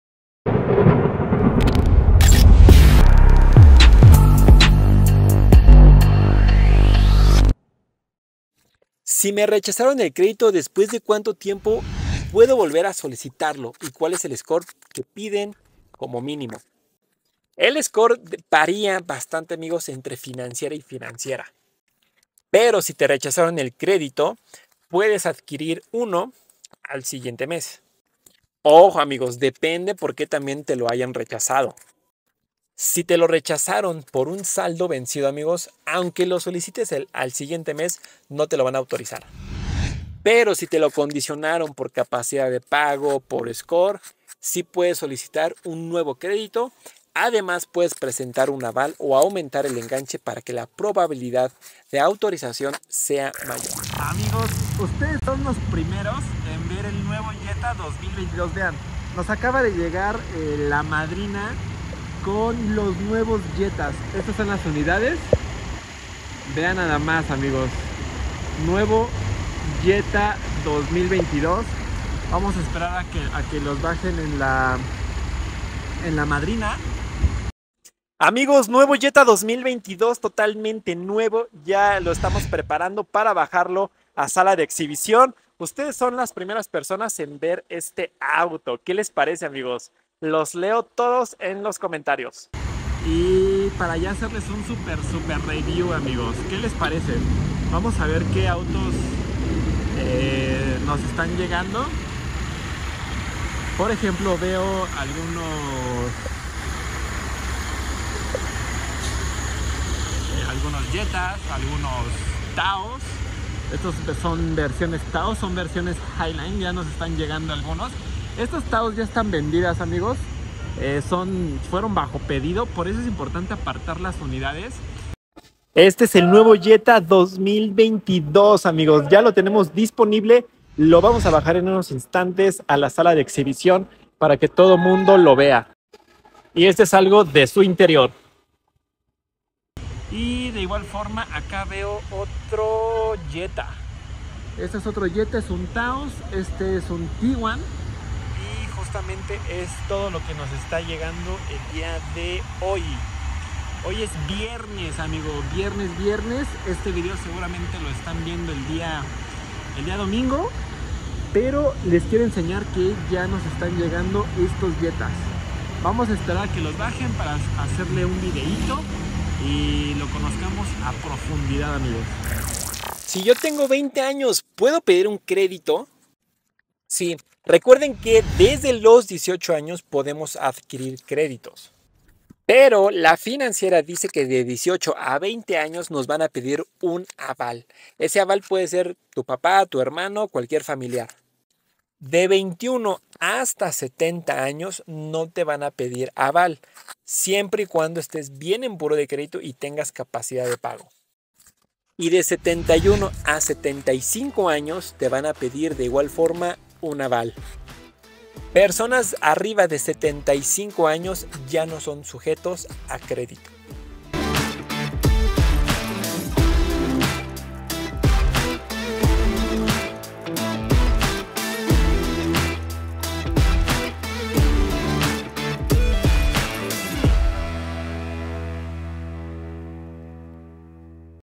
Así llegan los autos a la Concesionaria Volkswagen [KioKio]
Amigos!! es momento de estrenar u Volkswagen!